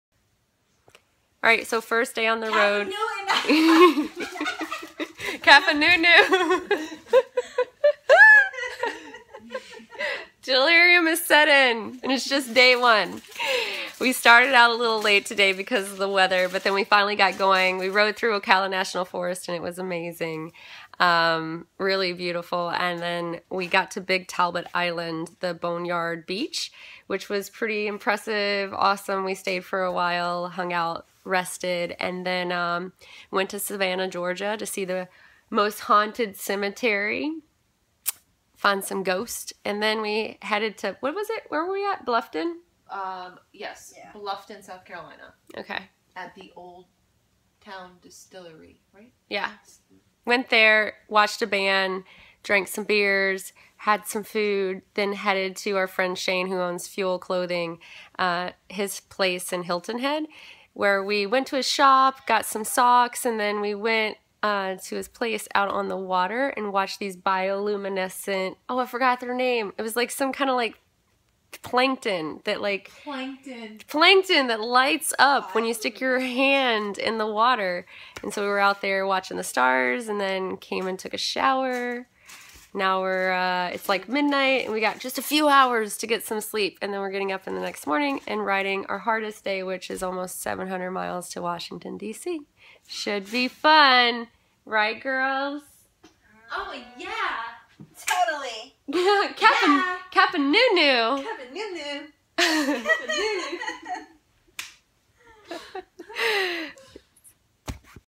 Northeastern Adventure Vlog Day 4
Daily mini videos recapping my month and a half journey on my motorcycle from Florida to New Hampshire, through the Adirondacks to Kentucky and  the Natchez Trace before heading back home to Florida. I will be crossing 19 states on my 2014 Harley Iron 883 on this trip.